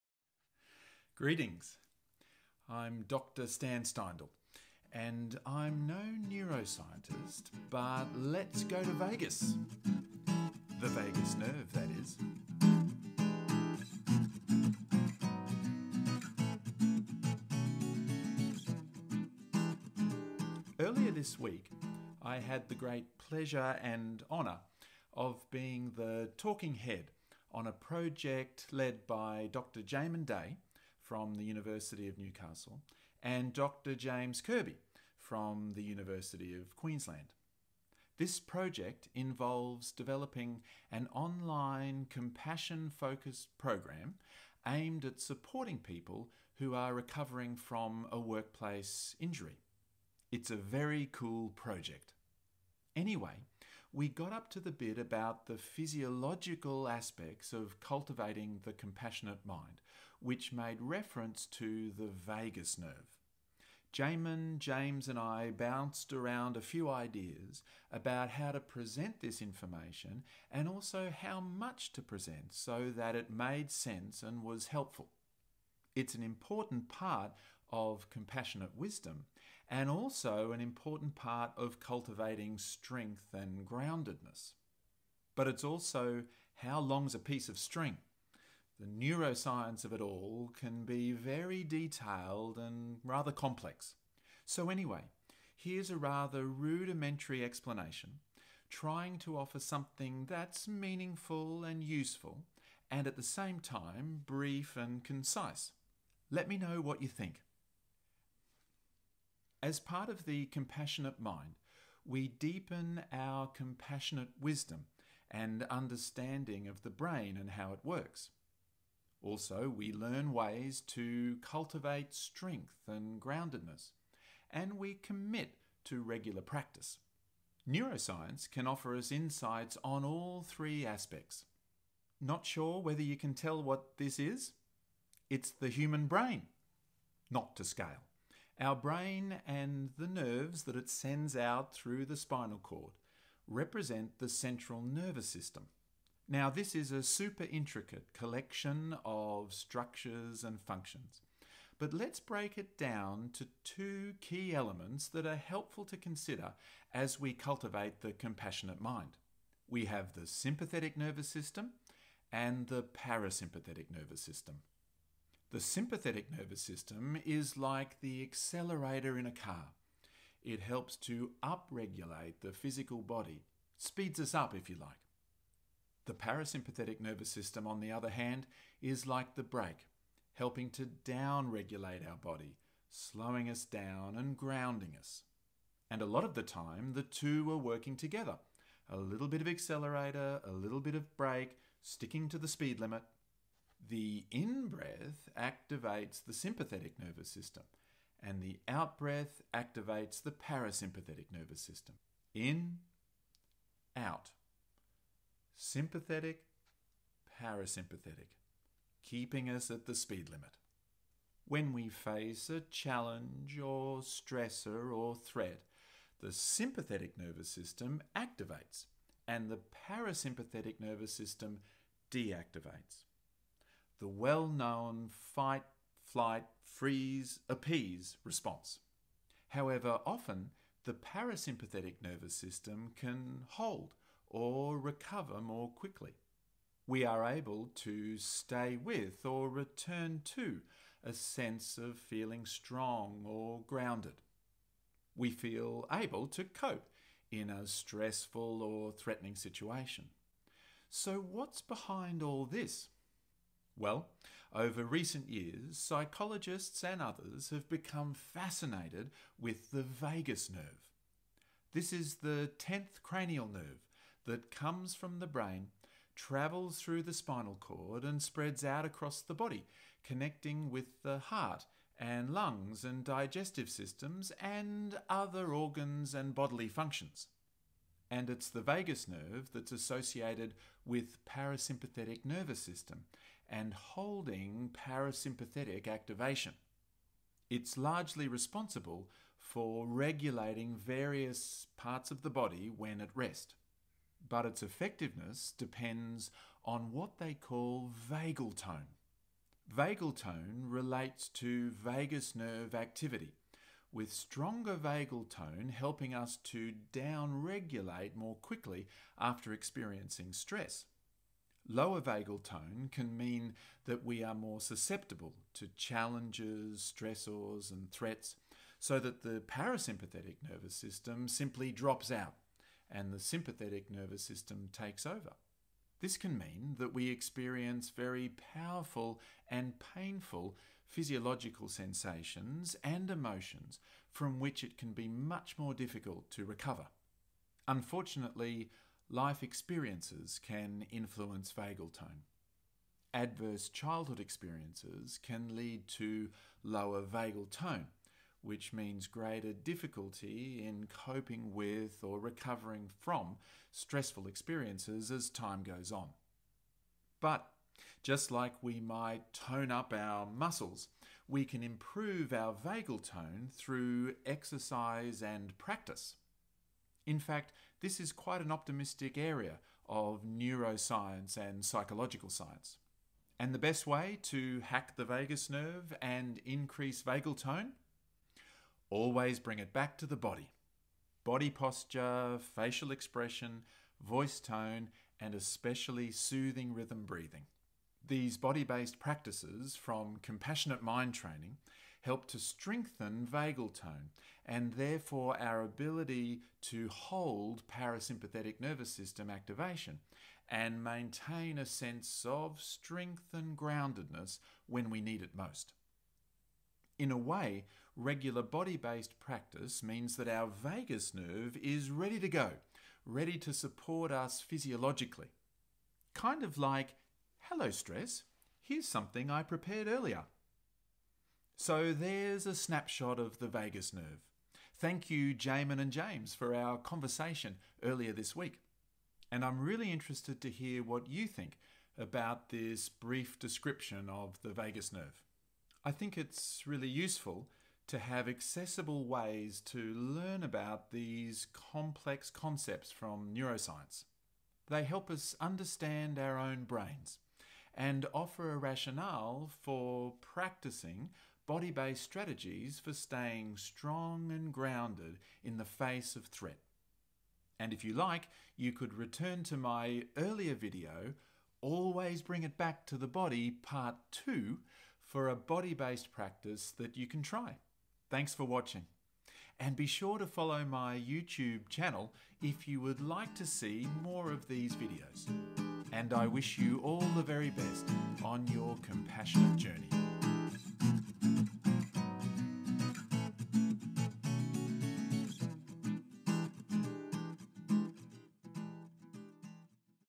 Let's go to Vagus! Vagus Nerve and Compassionate Mind
Having recently had the chance to discuss the role of the Vagus Nerve in compassionate mind training with Dr Jamin Day and Dr James Kirby, here is Dr Stan Steindl giving a brief and concise explainer of the brain, the sympathetic and parasympathetic nervous systems, the Vagus Nerve and the notion of Vagal Tone, and how to cultivate the compassionate mind.